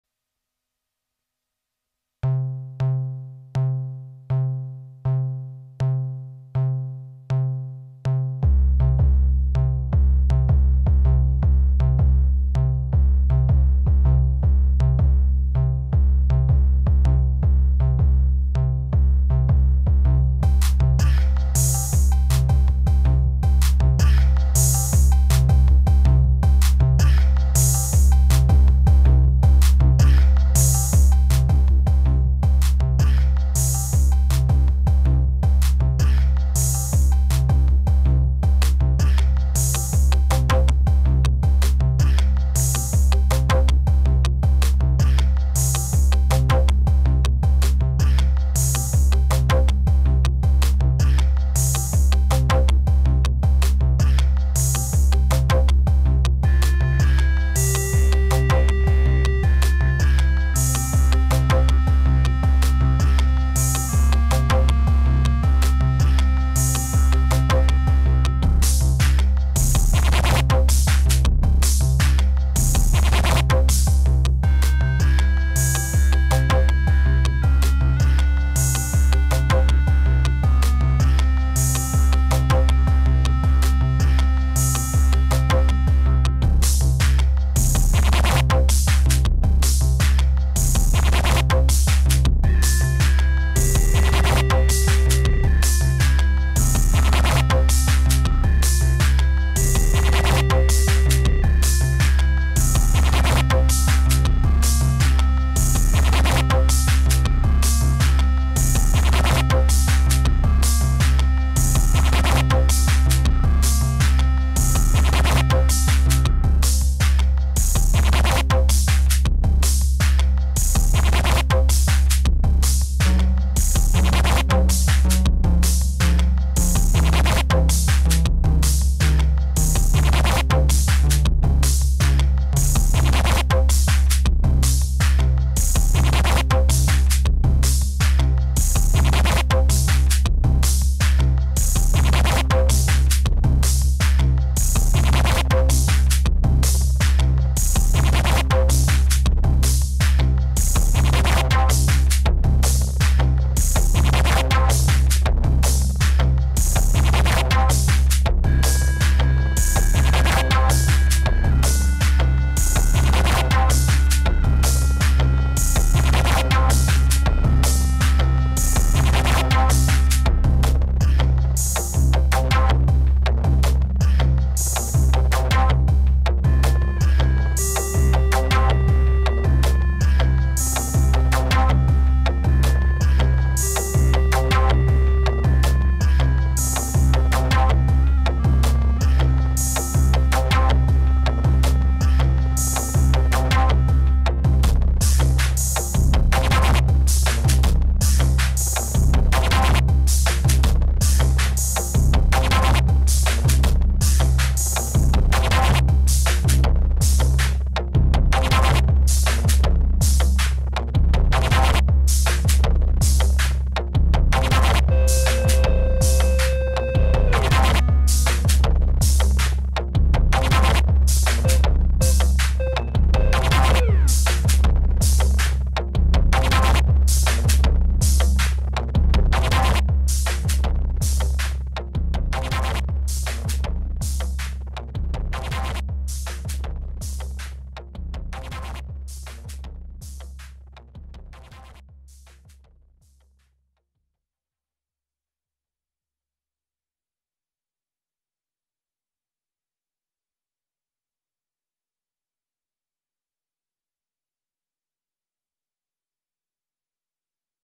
Political Mess
The HKSAR Government is suck!  Messed the whole city but irresponsibly blame the demonstrators.  When will she be shredded?

Korg Volca Set, with 2 Mix, 1 FM, 1 Modular, 1 Sample, 1 Drum and 2 Kick